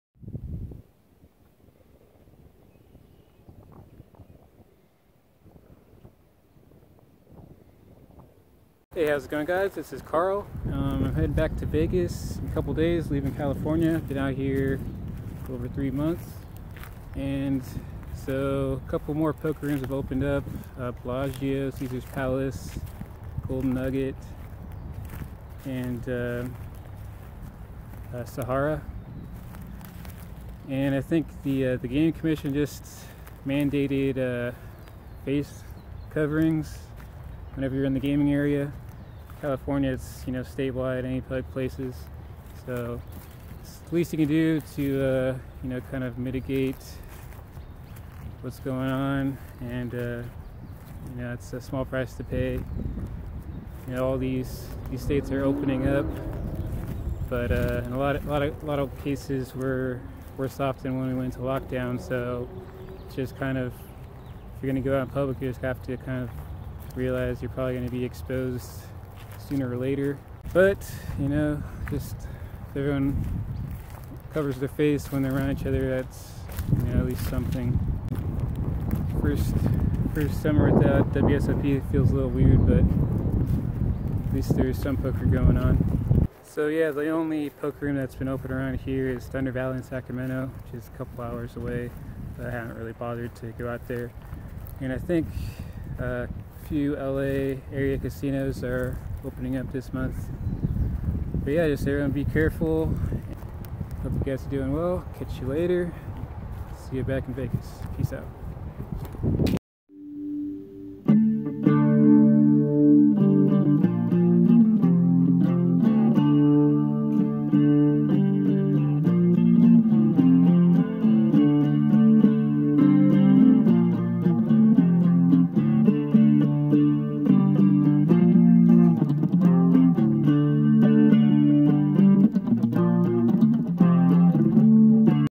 Vegas and California Poker Room Reopenings
A few more poker rooms on the west coast are operational with added precautions.